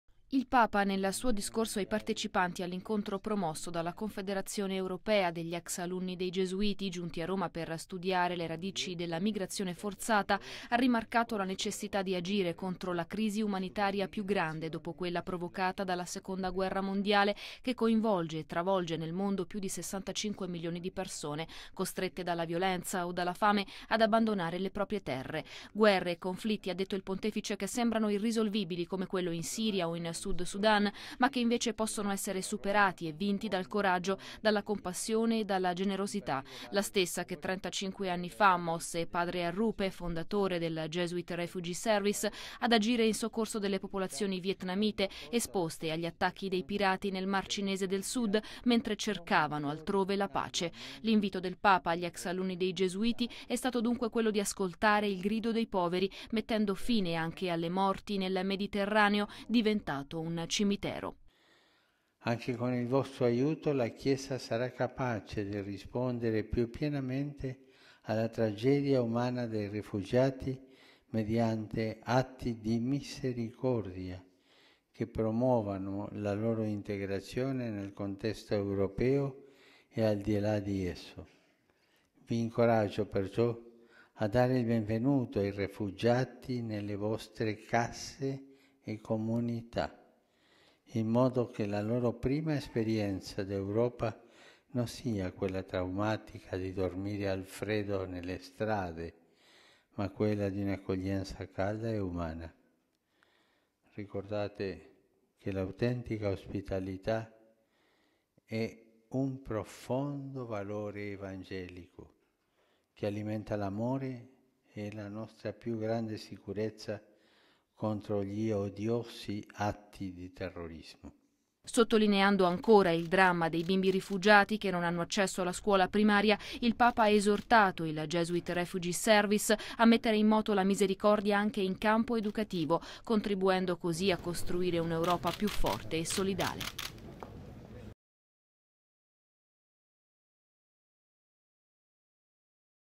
Udienza ex Alunni dei Gesuiti 17.09.2016
Ospitalità e misericordia contro il dramma dell’immigrazione

Papa Francesco incontrando gli ex alunni dei Gesuiti ha rimarcato il suo dolore per l’immane tragedia che coinvolge i migranti e ha chiesto atti di misericordia e accoglienza per mettere fine alle morti in mare e al terrorismo.